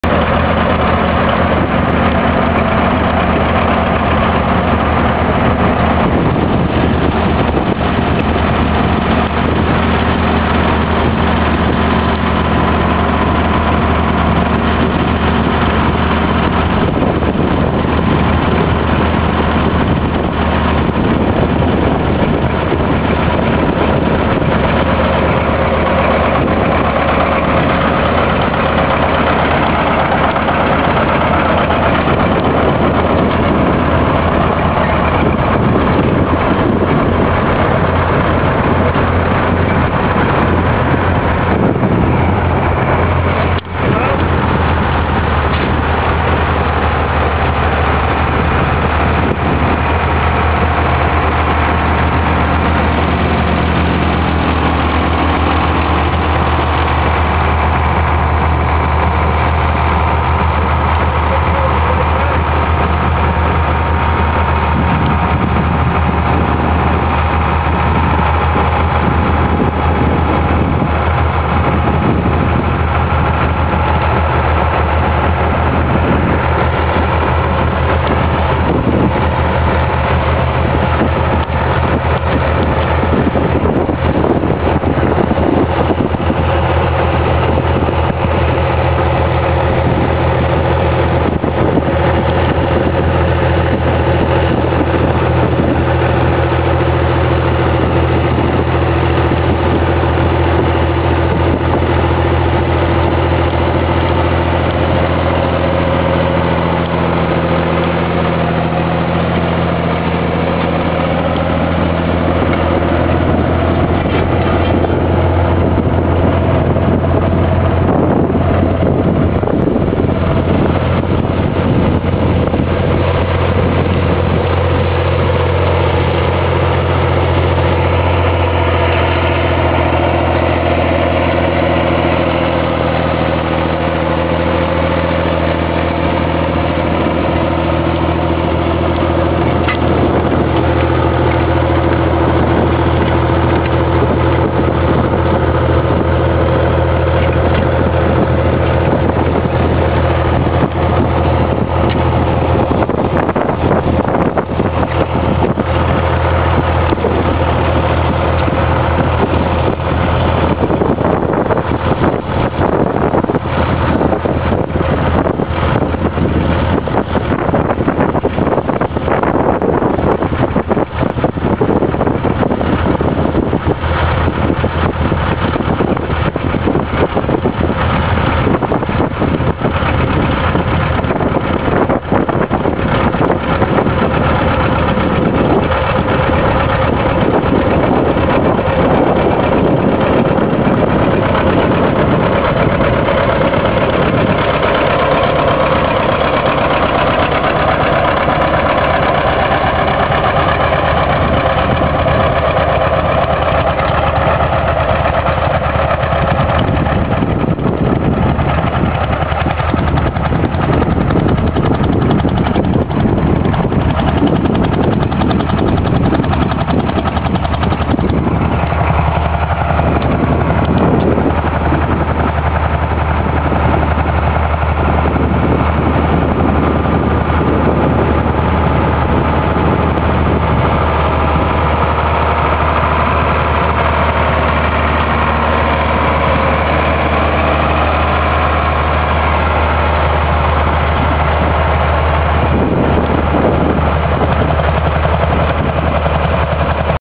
working day at kale farm hayle vintage tractors messrs thomas and marks
vintage john deeres owned by the thomas and bawden familys  at working day in the memory of bobby pellow